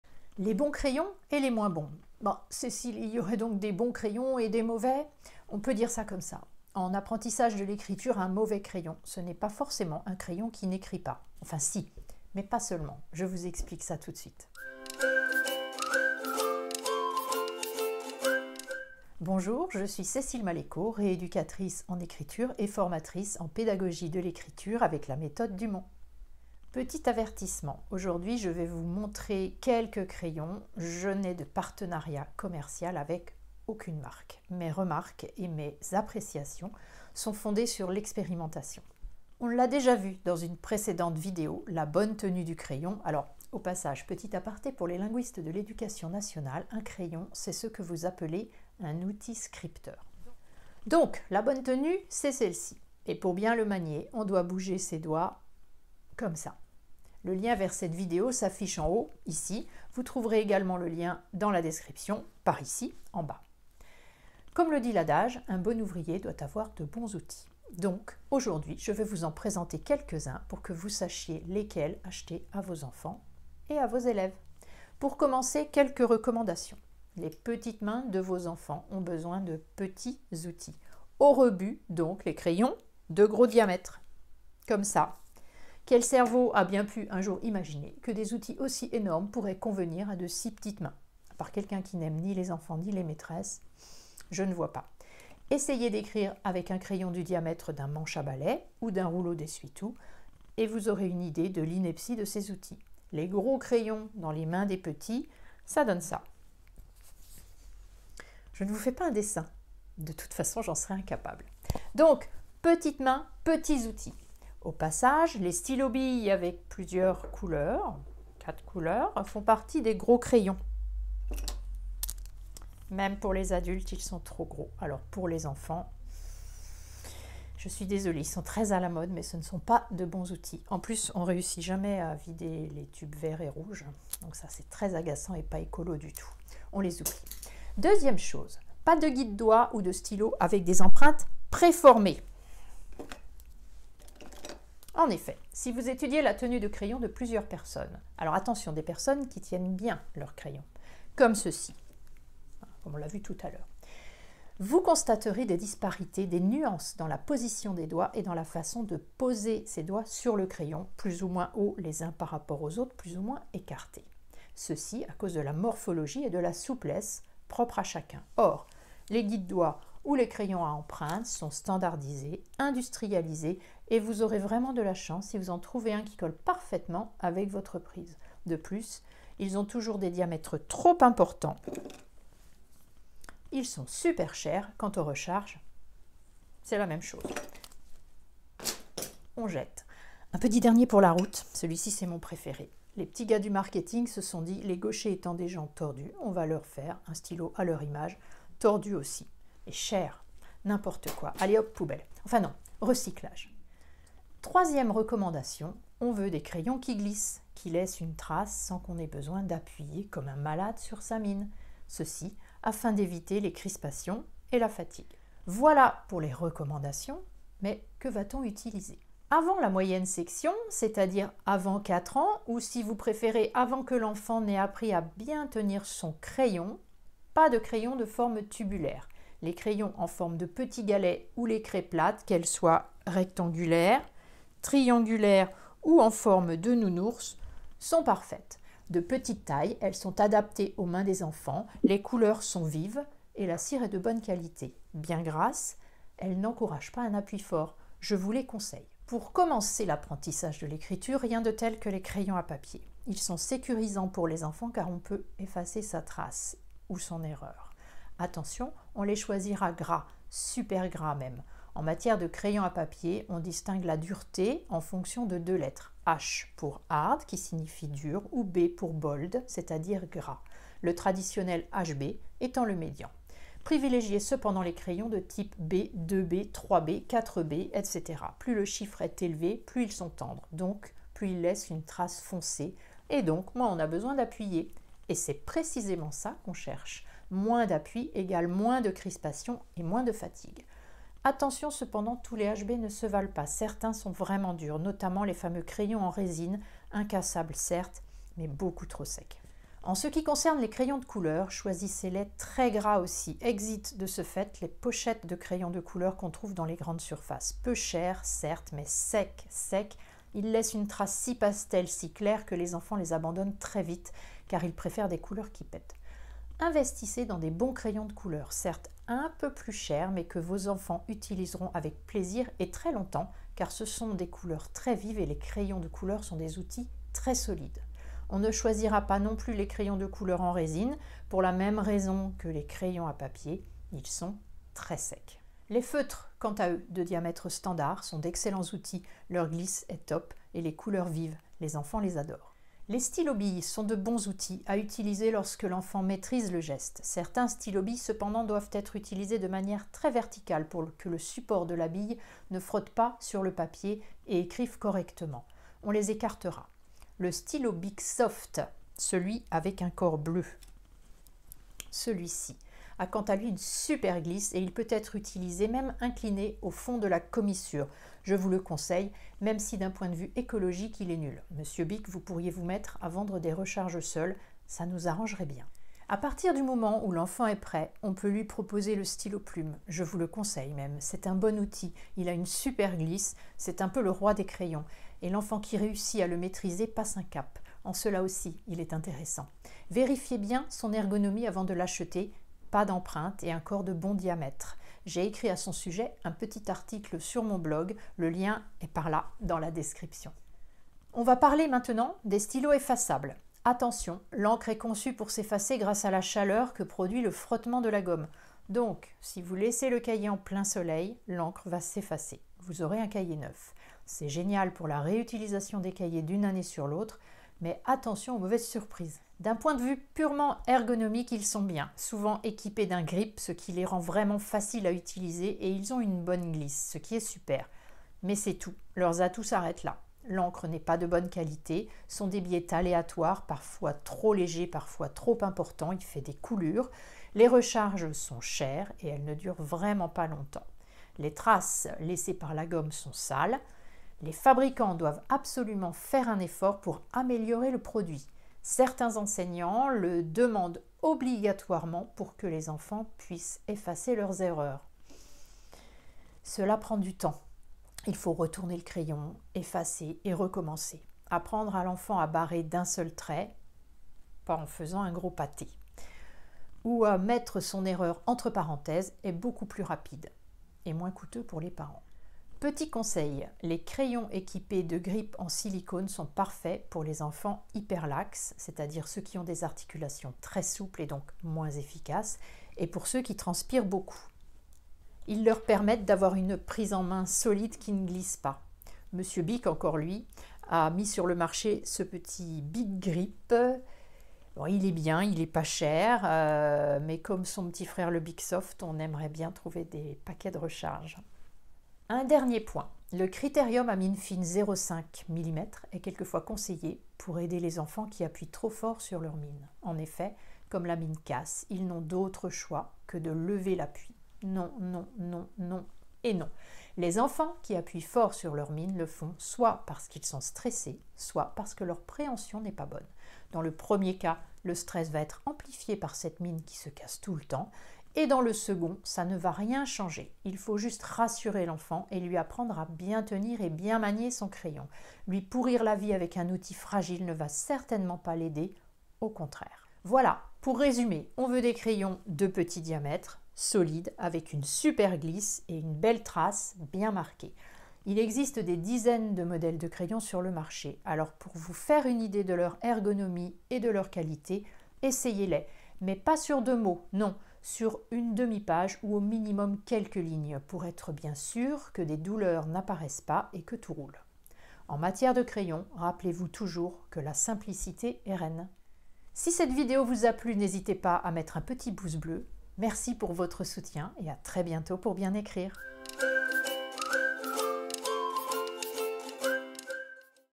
Les bons crayons… et les moins bons !
Pour bien écrire, le choix des crayons est capital ! Découvrez quelles sont les qualités d'un bon crayon et surtout apprenez à choisir les bons crayons pour vos enfants ou élèves…
• Les craies pour les petits qui n'ont pas encore appris à se servir d'un crayon :
  - en forme de galets : Crayons Rocks
  - rectangulaires : Stockmar
  - triangulaires : Primo
  - En forme de tête d'ours : Jovi